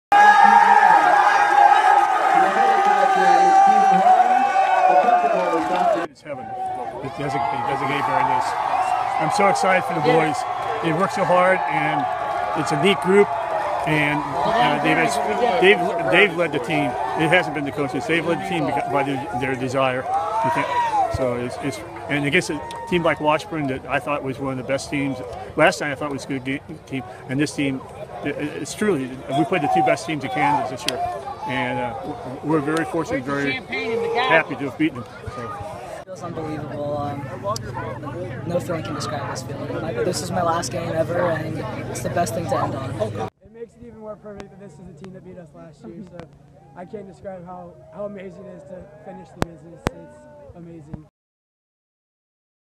Blue Valley Northwest wins Kansas Class 6A boys soccer title
The Blue Valley Northwest boys soccer coach and players react to defeating Washburn Rural 3-2 in penalty kicks to win the Kansas Class 6A state championship on Saturday.